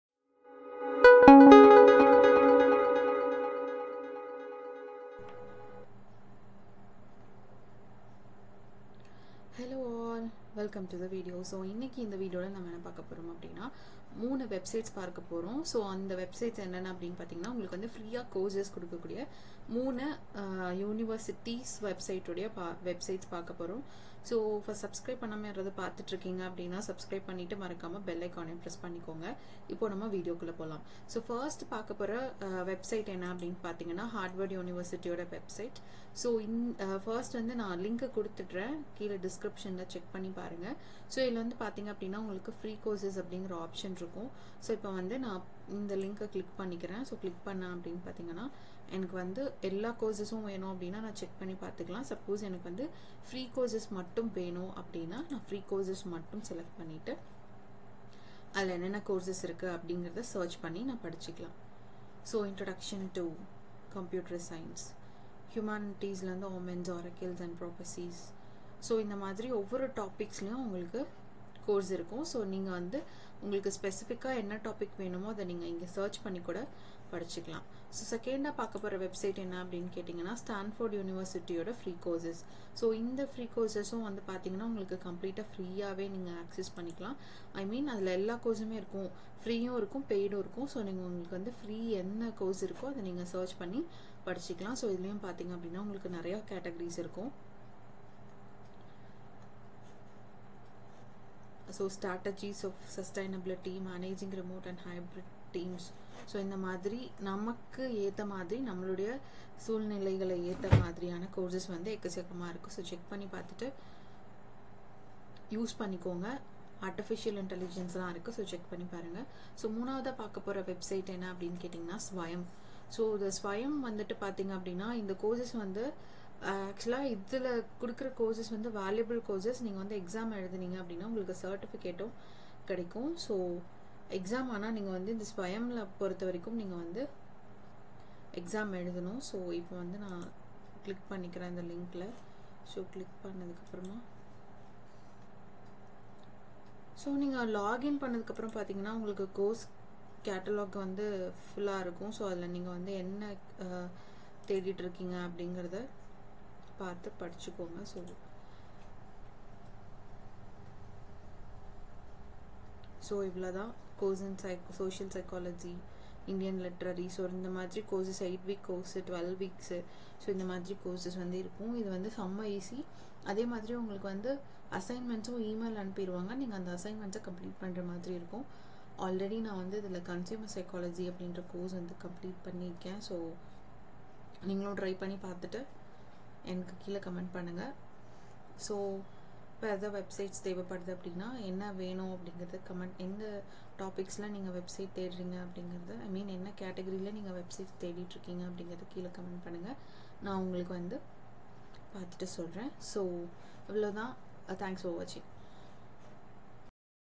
Top universities offer free courses. Learn now. Unlock your Knowledge
Embark on a remarkable educational journey with our curated collection of free courses offered by the world's top universities. In this captivating video, we unveil an abundance of opportunities to expand your horizons, enhance your skills, and delve into diverse disciplines, all from the comfort of your own home.